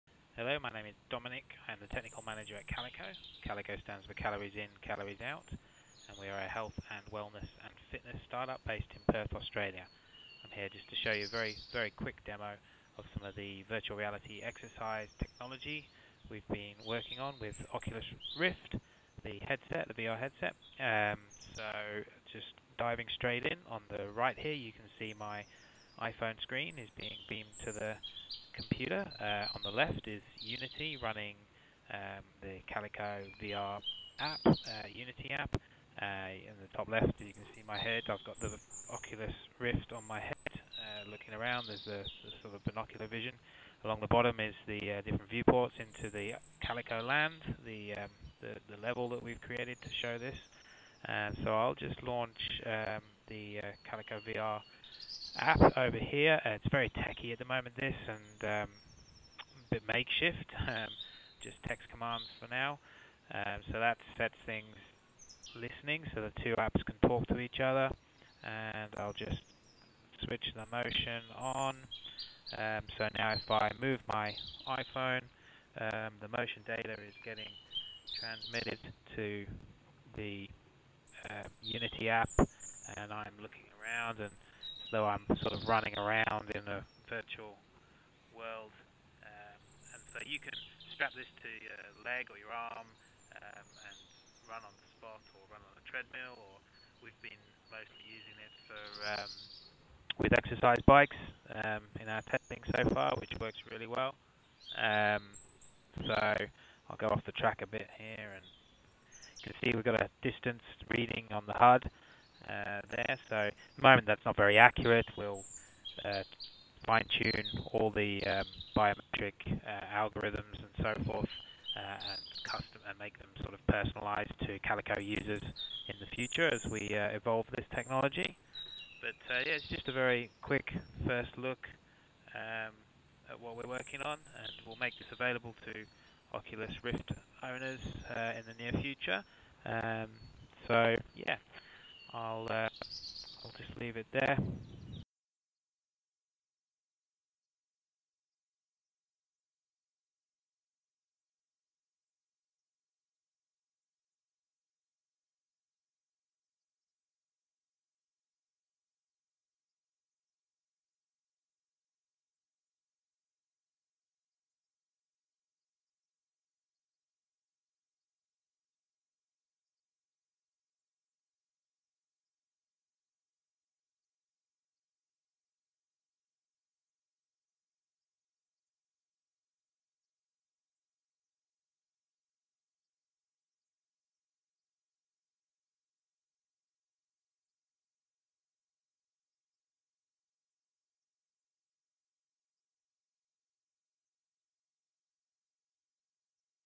Calico app - Oculus Rift. Home exercise virtual reality demo.
An early demo of our virtual reality exercise technology that combines Oculus Rift and iPhone motion sensors for a unique fitness experience.